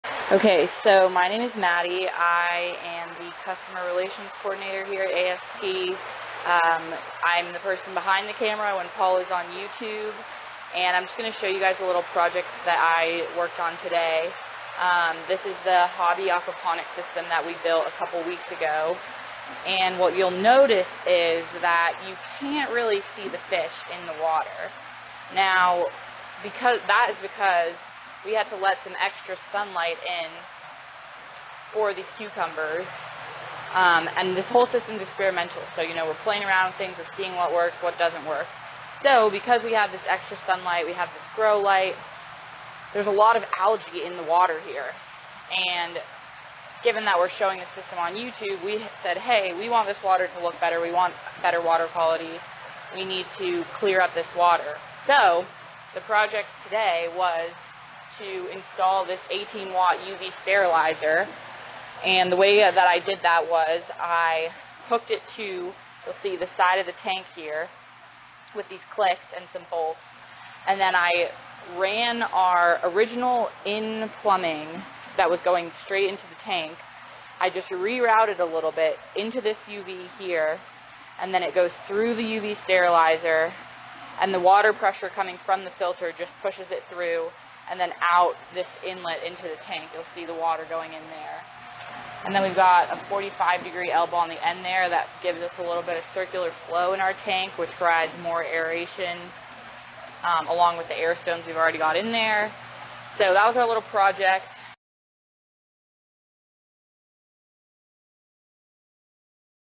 Adding UV Sterilizer for Better Water Clarity
Paul will be back next week, but follow up today with Maddie as she shows the small aquaponics system and the UV sterilizer she added to improve water clarity.

While a UV light doesn't tend to be necessary for basic fish health (occasionally longer residence times can be used to kill pathogens), they are excellent at killing floating algae or greenwater in koi systems.

UVs are not required in aquaponics, many people avoid them. They are being utilized here because we are showcasing this system on youtube and would like viewers to be able to see the fish.  Shadecloth over tanks can also be used to reduce algae buildup.